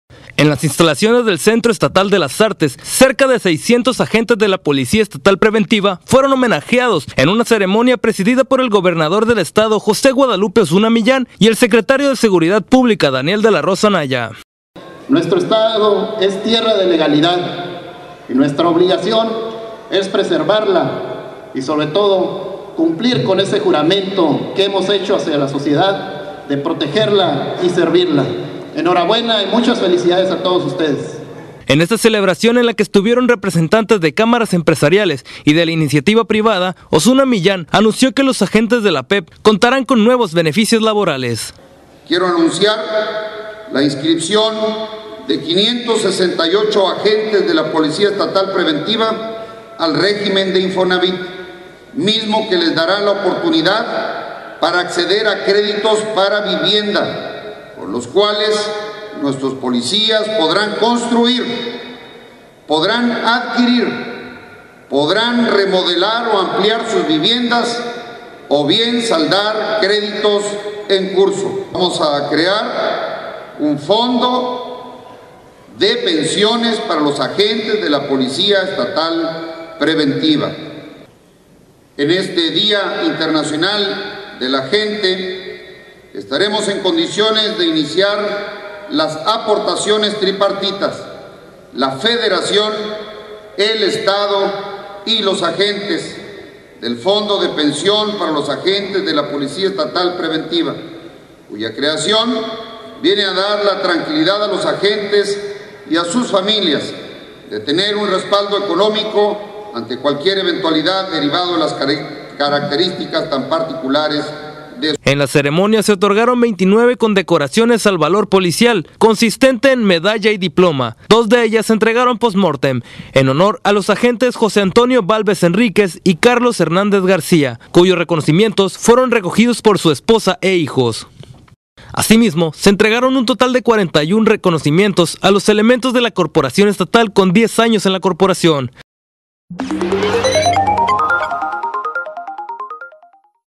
Celebra PEP día del policía con dos beneficios laborales nuevos
En las instalaciones del Centro Estatal de las Artes, cerca de 600 agentes de la Policía Estatal Preventiva fueron homenajeados en una ceremonia presidida por el gobernador del estado, José Guadalupe Osuna Millán y el secretario de Seguridad Pública, Daniel De la Rosa Anaya.